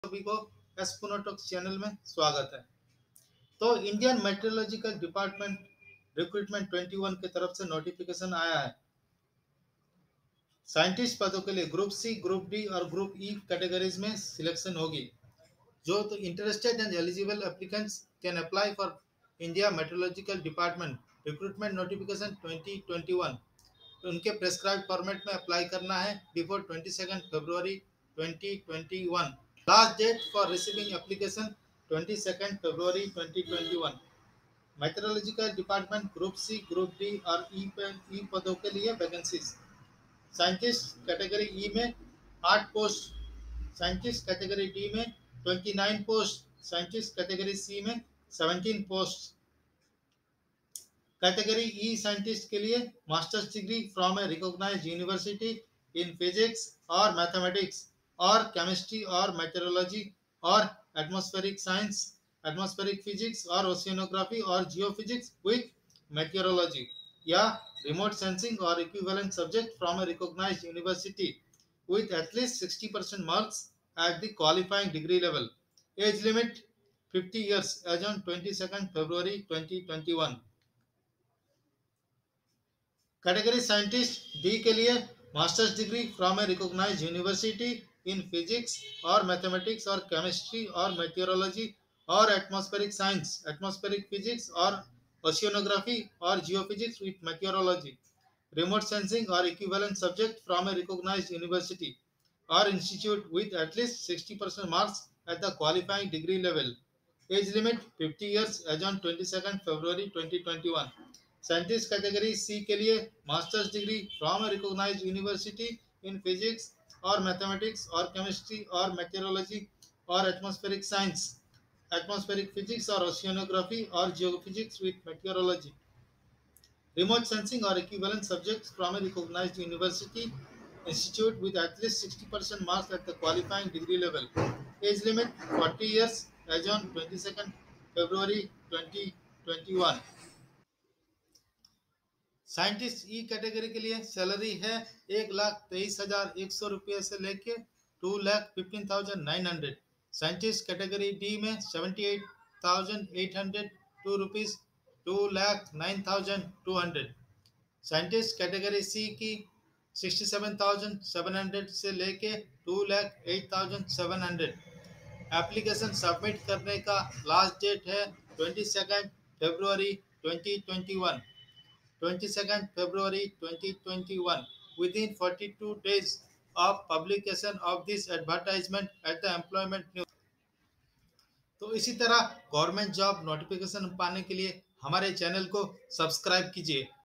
IMD Recruitment 2021 I Government jobs 2021 I Sarkari Naukri 2021 IVacancy at Meteorological dep
spunotech
IMD Recruitment I Government jobs 2021 I Sarkari Naukri 2021 ,
IMD Recruitment 2021 | Scientist C, Scientist D & Scientist E Posts | 54 Vacancies | Last Date: Within 42 days | Apply Online @ imd.gov.in

IMD invites online application from qualified persons to fill up the post of Scientist ‘C’, Scientist ‘D’ & Scientist ‘E’ posts. As per recruitment notification of IMD, the weather department have 54 live vacancies for said posts. IMD has decided to fill up the requirements in following areas such as Forecasting, Instrumentation, Computer/ Information Technology (IT) & Agriculture Meteorology. IMD is going to recruit candidates on direct recruitment basis. Engineering job seekers use this opportunity to join in weather department jobs